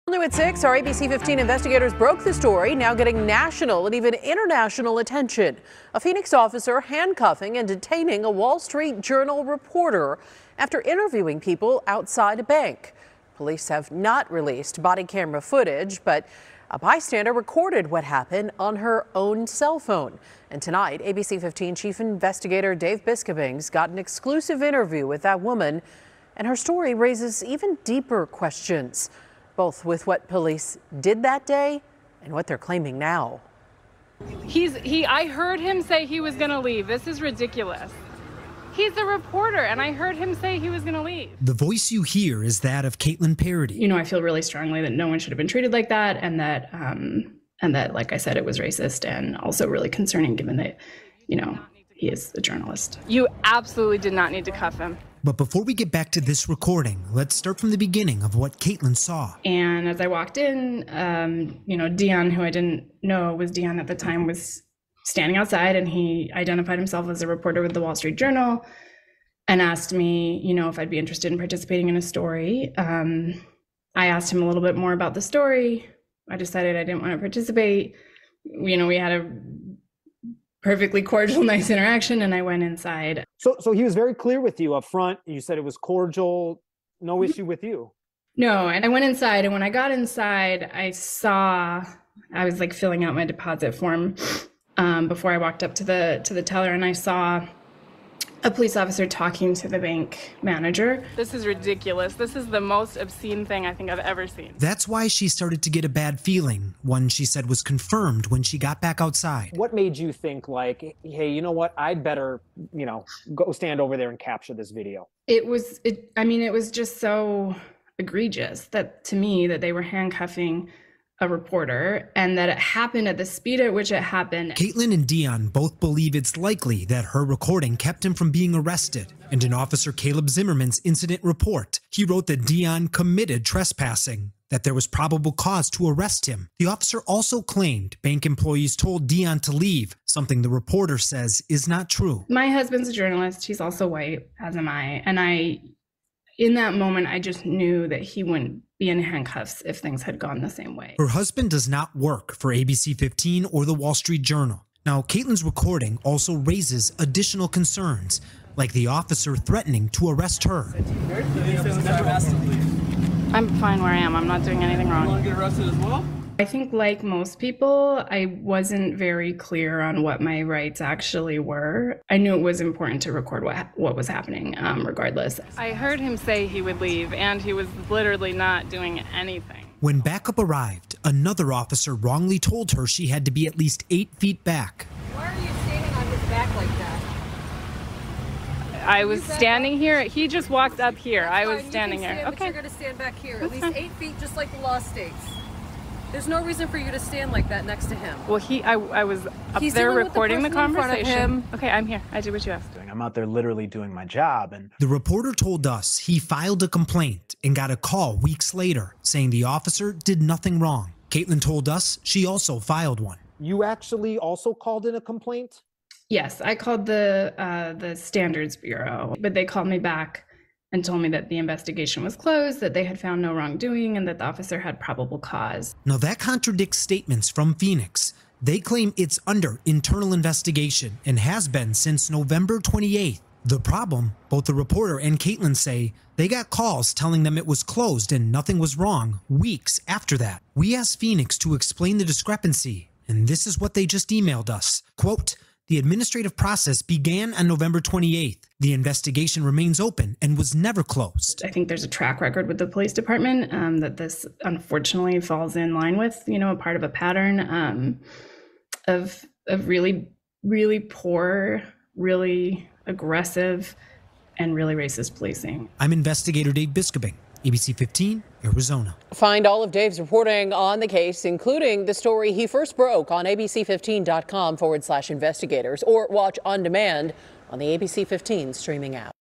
Witness explains why she recorded officers actions
A witness is speaking out explaining why she decided to record a Phoenix incident where police officers placed a working reporter in handcuffs.